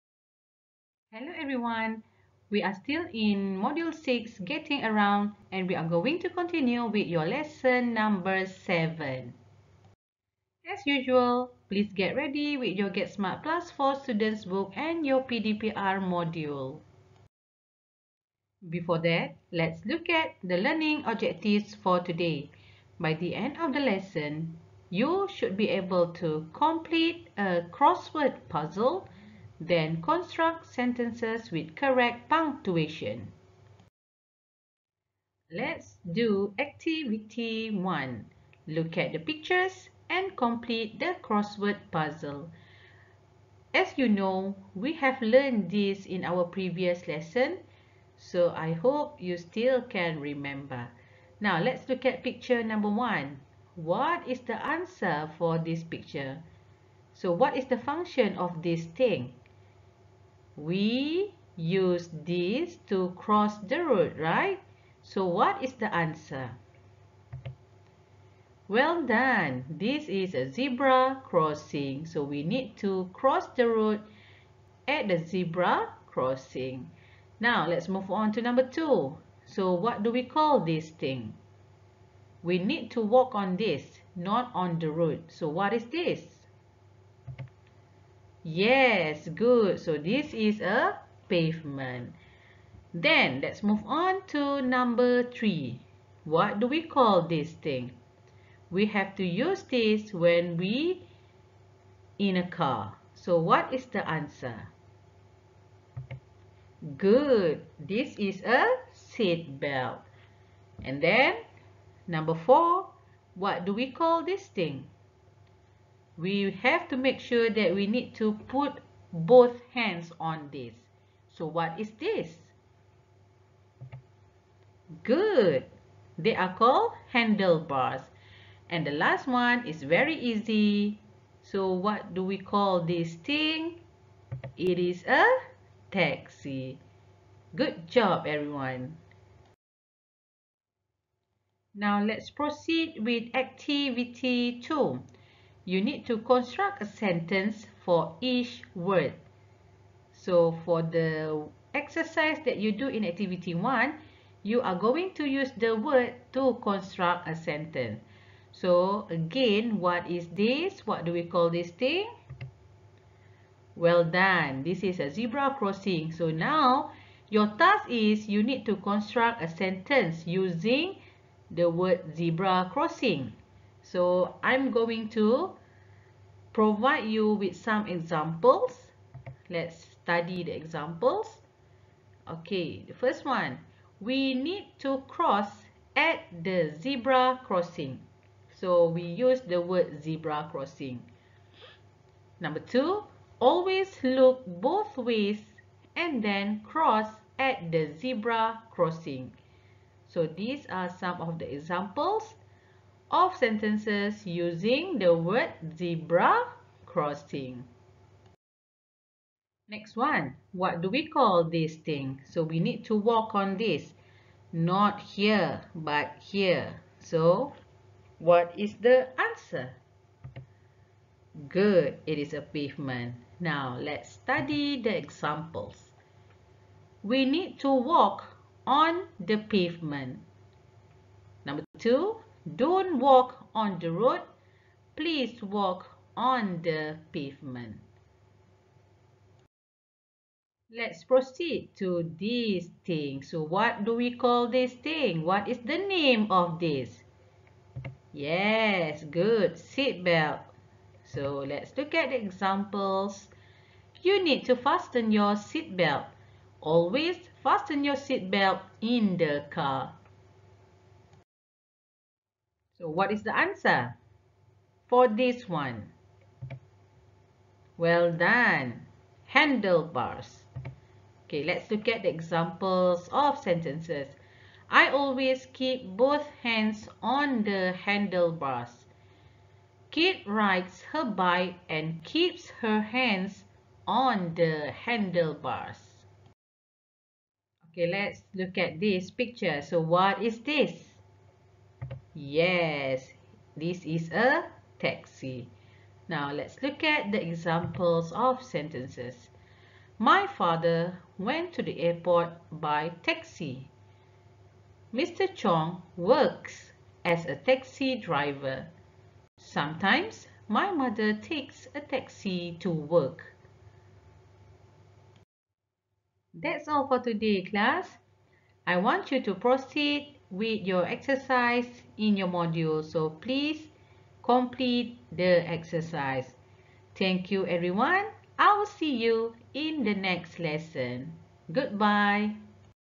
GET SMART PLUS 4 I MODULE 6: GETTING AROUND I PAGE 148
LESSON 7 MODULE 6
By the end of the lesson, pupils should be able to:
- complete a crossword puzzle.
- construct sentences with correct punctuation.